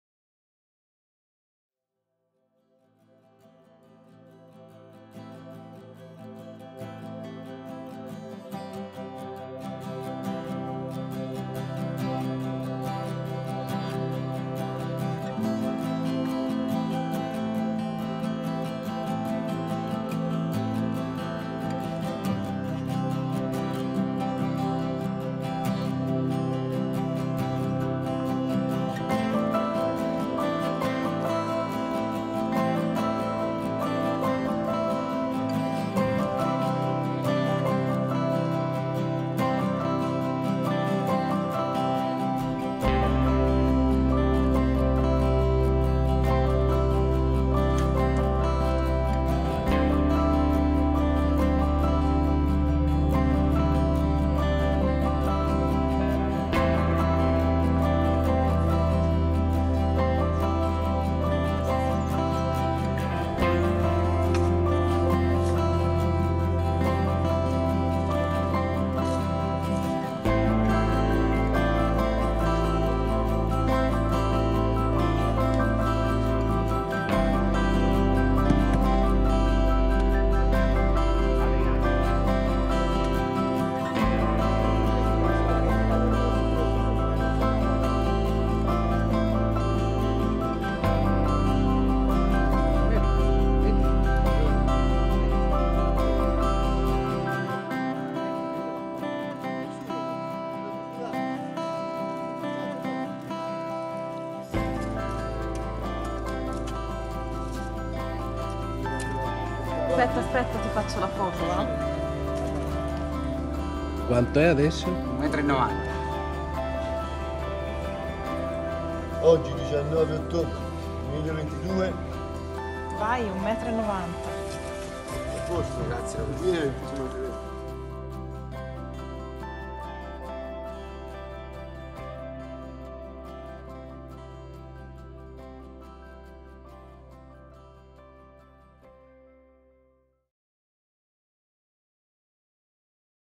Omaggio a DANTE Piantumazione Cipresso Basilica di Santa Croce 19 10 22
Firenze, 19 ottobre 2022.
L'eccezionale piantumazione del Cipresso-Omaggio a Dante Alighieri per il progetto "All'Ombra de' Cipressi - Firenze in memoria dei Poeti" dell'Affratellamento.

Progetto del Teatro Affratellamento, realizzato con il sostegno della Fondazione CR di Firenze e la preziosa collaborazione da parte di personalità del mondo della Cultura, Enti e Istituzioni, tra cui: il Comune di Firenze; L’Opera di Santa Croce; Museo delle Cappelle Medicee; l’Accademia della Crusca; il Conservatorio Luigi Cherubini; il Comune di Scandicci, Cimitero degli Inglesi, la Pieve di Badia a Settimo...